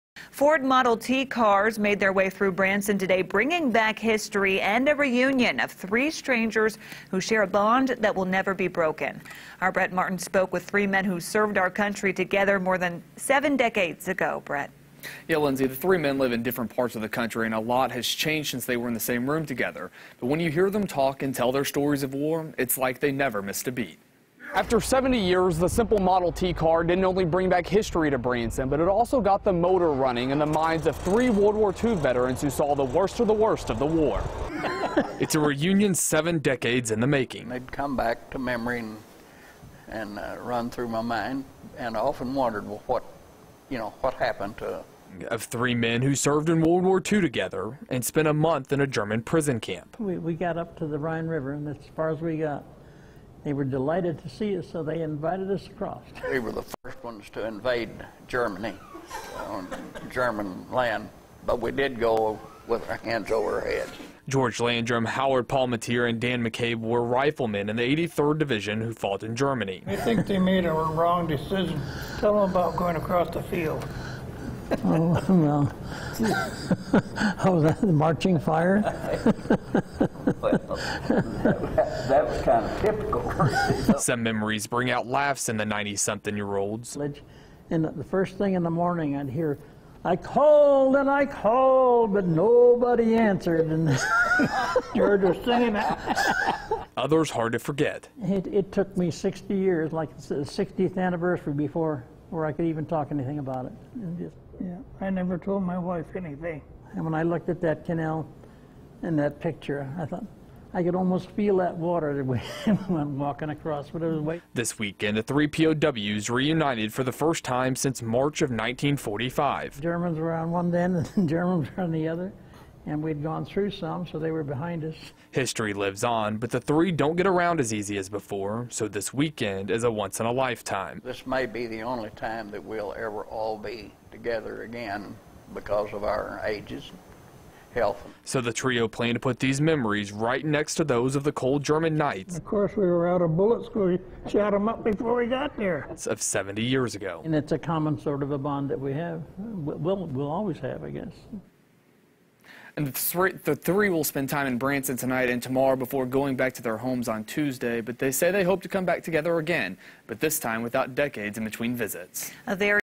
WWII Branson interview
3 Battle of the Bulge Veterans talk about their time in WWII